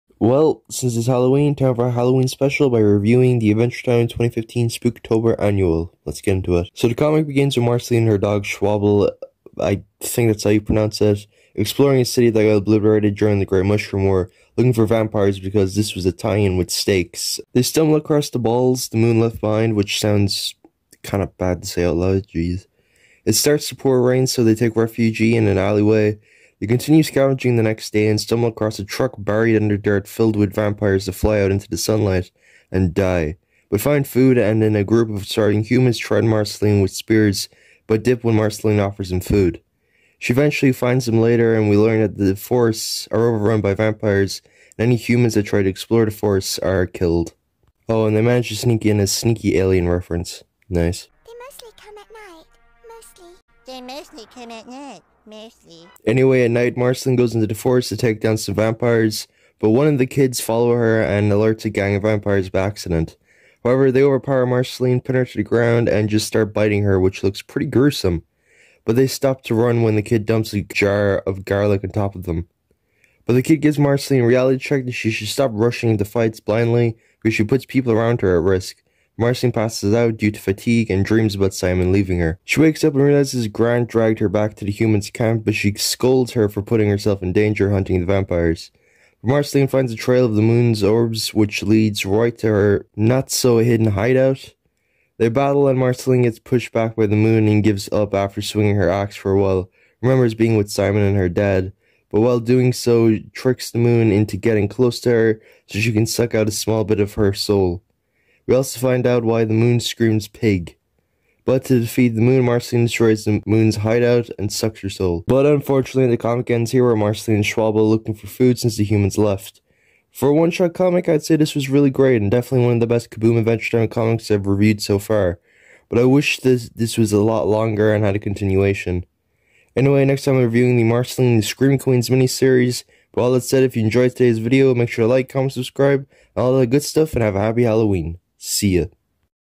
Adventure Time Spooktober 2015 Annual Review (Happy Halloween! 🎃👻)
Since it's Spooky season (and the last day of it) let's take a look at the surprisingly good 2015 Spooktober Annual that tied in with the Stakes Miniseries!